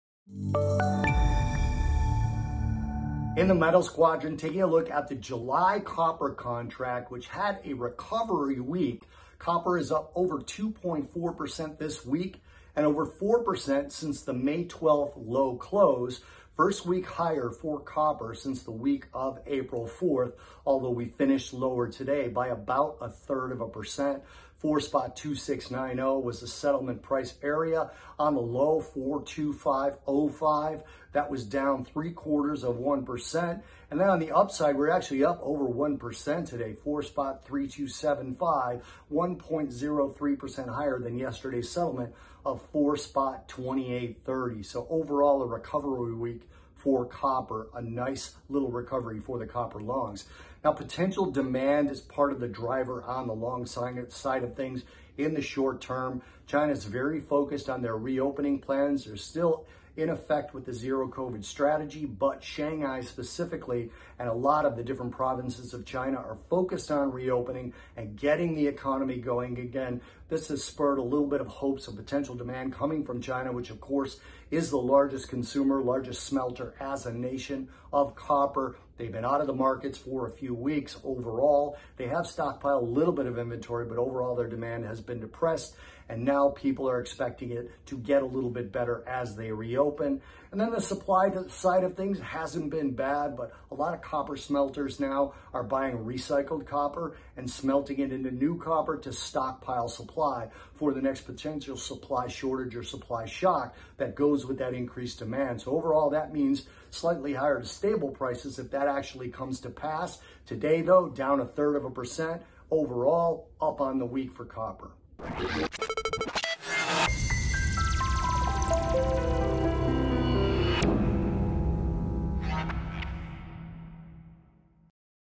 Metals Market Commentary: Bob Iaccino, 5/20/22
Copper rallies more than 2.4% this week. Bob Iaccino discusses.